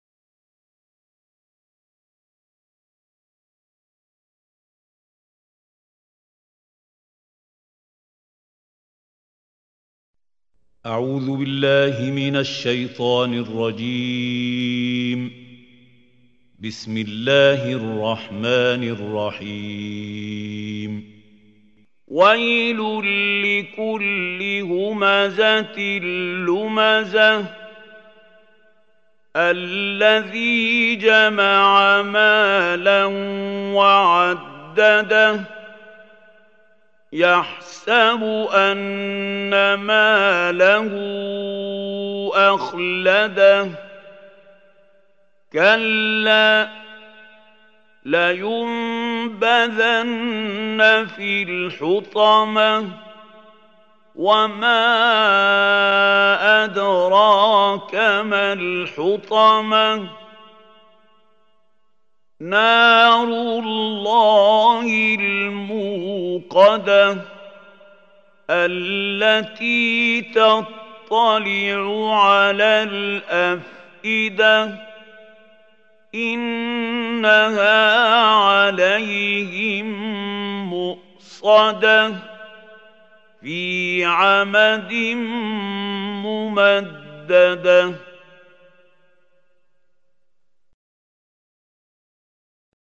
El coran en Español: 104. Sura AL HUMAZAH (LOS DIFAMADORES) : Traducción española (latinoamericano)
Sura AL HUMAZAH (LOS DIFAMADORES)  Esta sura fue revelada en el período temprano de La Meca. Consta de 9 aleyas.